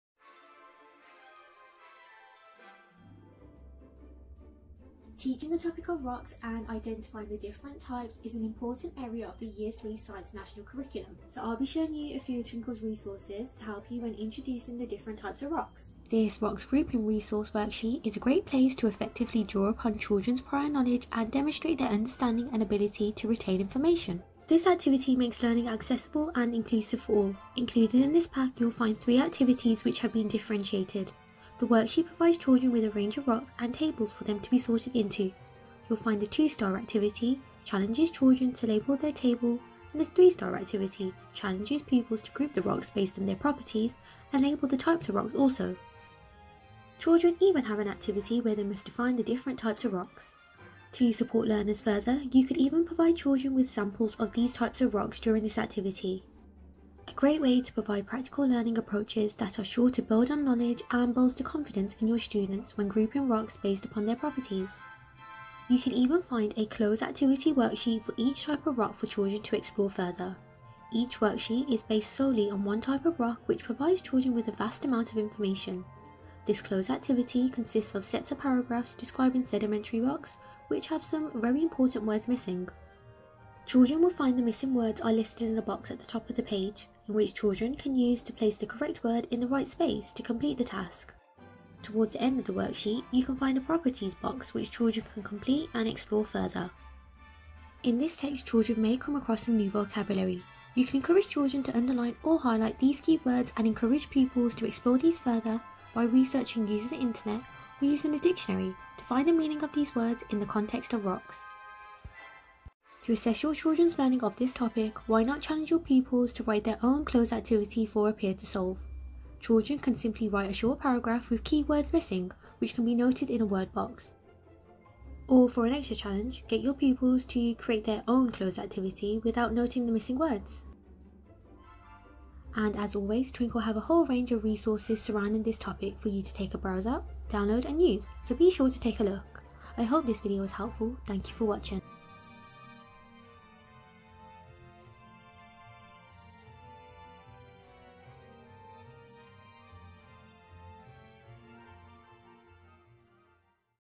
Teaching Your Children All About Rocks | KS2
In this video, Twinkl Teacher Saleena showcases a range of rock resources to support children identify the different types of rocks by comparing and grouping them using our Grouping Rocks Worksheet:

Use this differentiated rocks worksheet pack with your children, so they can effectively draw upon their prior learning and demonstrate their understanding and ability to retain information.

0:00 Introduction
0:16 Grouping rocks activity
1:06 Rocks Cloze Activity
1:38 Ideas how you can challenge children further
2:13 Twinkl's whole range of rock resources
2:23 Outro